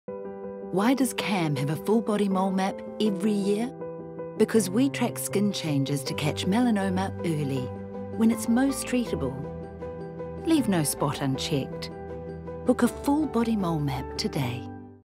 MoleMap Spot My Reason (15" - October 2019)
Find out here why Kiwis choose a Full Body MoleMap each year. A Full Body MoleMap means full body reassurance. At MoleMap, we thoroughly check every spot on your body, from head to toe and create a photographic map of your skin. This means we can track any changes over time and detect melanoma early... when it’s most treatable. Leave no spot unchecked – book a Full Body MoleMap today.